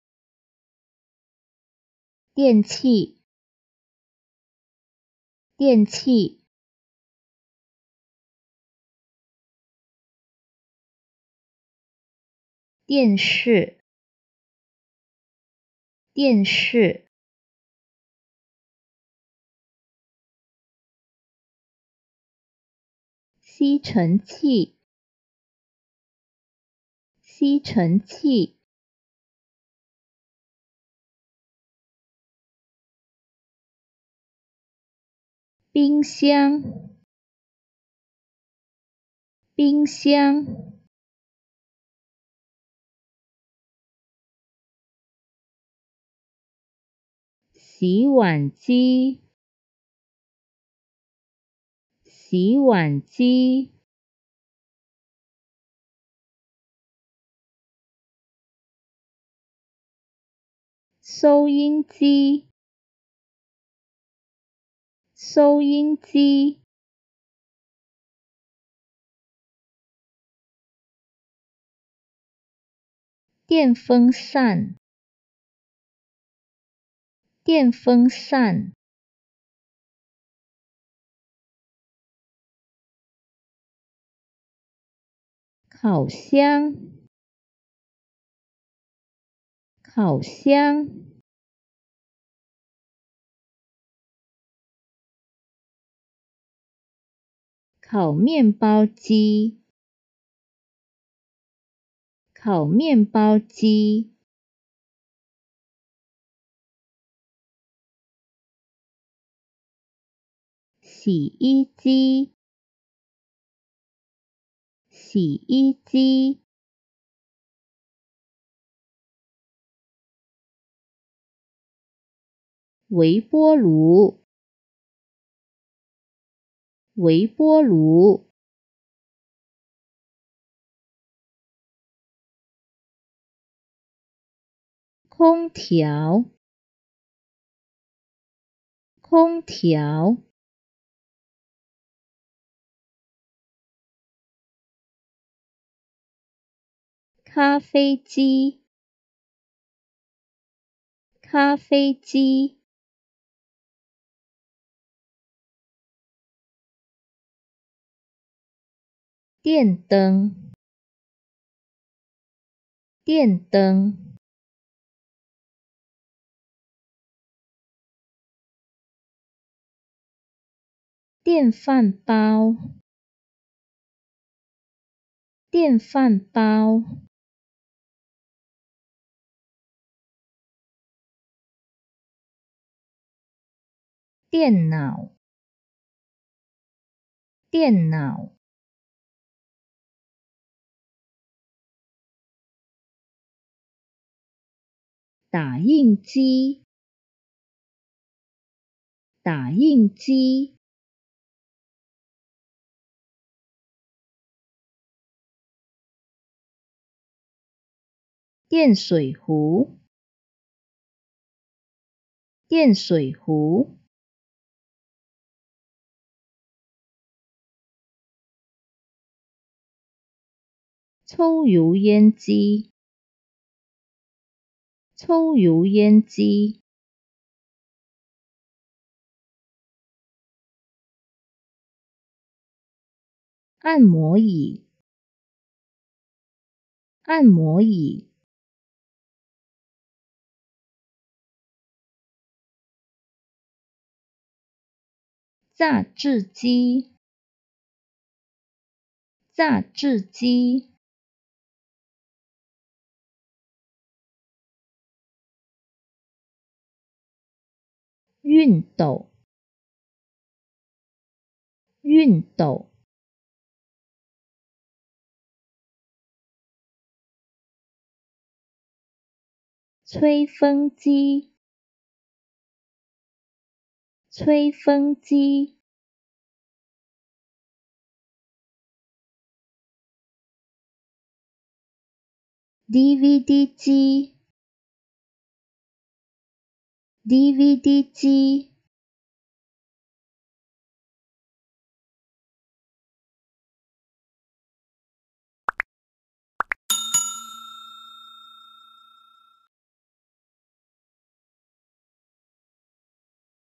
Electrical Appliances in Chinese Mandarin 【 学中文 – 电器 】
Learn how to say Electrical Appliances in Chinese.
今天和大家分享如何用中文说出我们常用的电器

“Change is the end result of all true learning.”